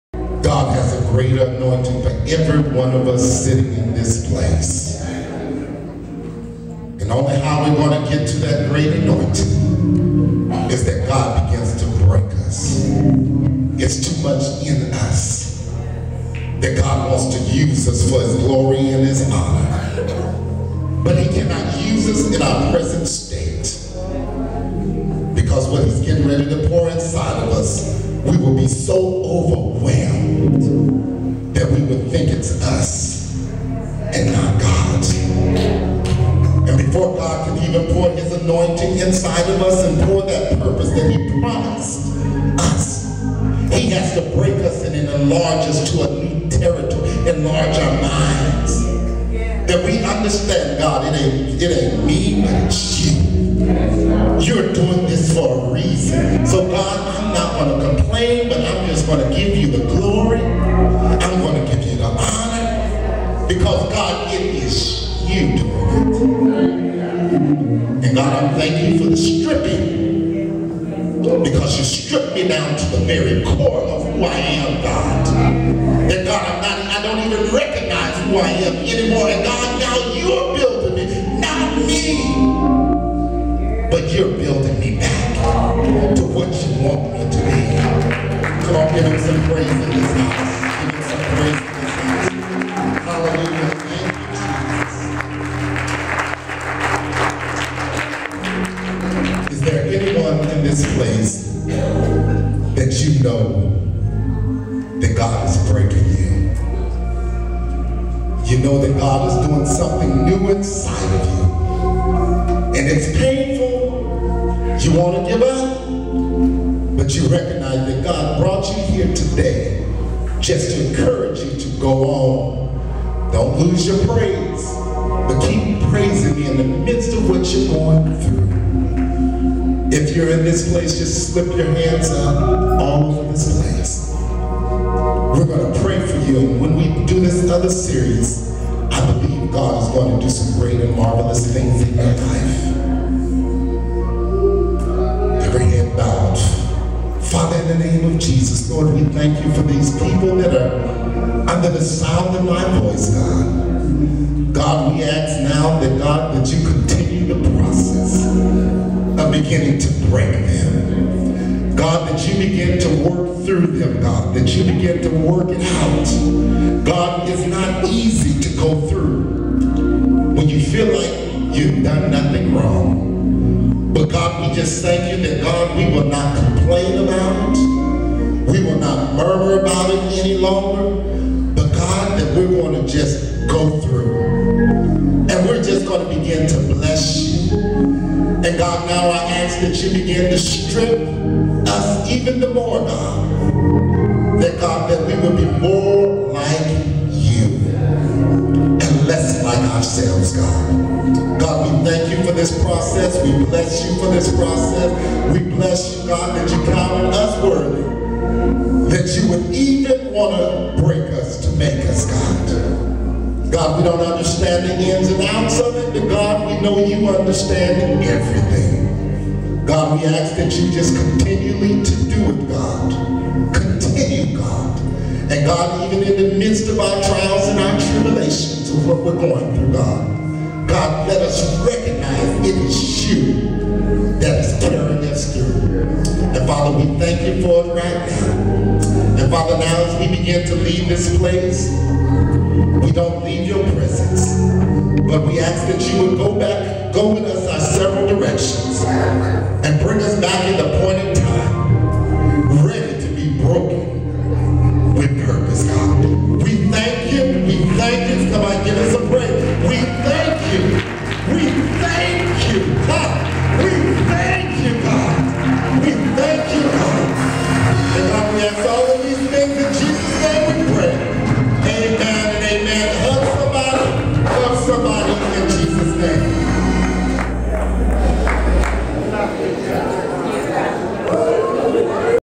"Make Me Over Again" (Part 3 of 3) (February Series Week 4 of 4)
Bishop Wade E. Harris
~Feb. 22, 2015~
"Morning Service"
Seed of Faith Ministries
1230 Winter St. NE Salem, Oregon 97301
(On the corner of Market St. NE & Winter St. NE)